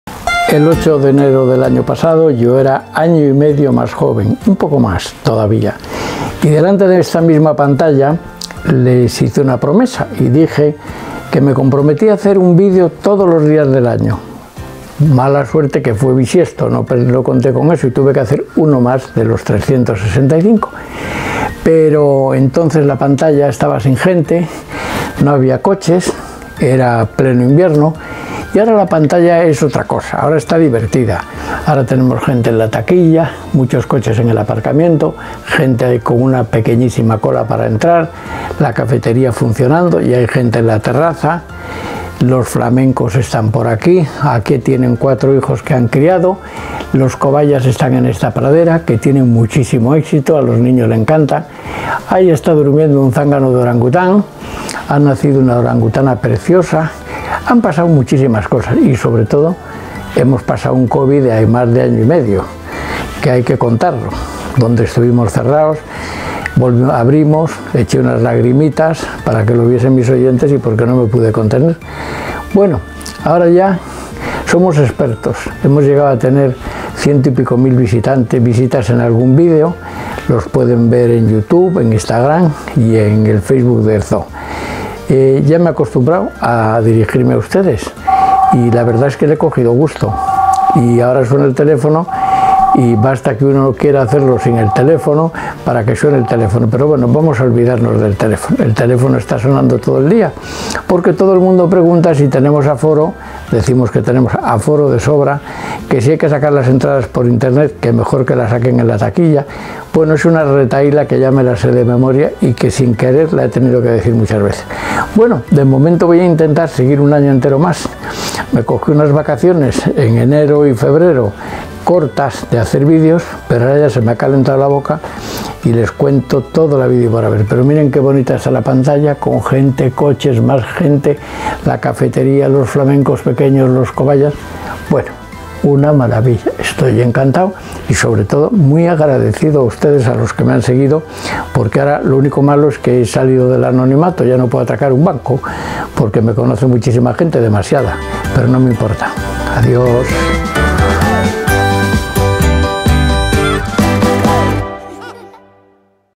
¡Programa número 500!
Hoy, José Ignacio Pardo de Santayana celebra los 500 programas que lleva ya grabados desde el zoodesantillana. ¿Quieres saber más? ¡No te pierdas el vídeo de hoy y ventealzoodesantillana!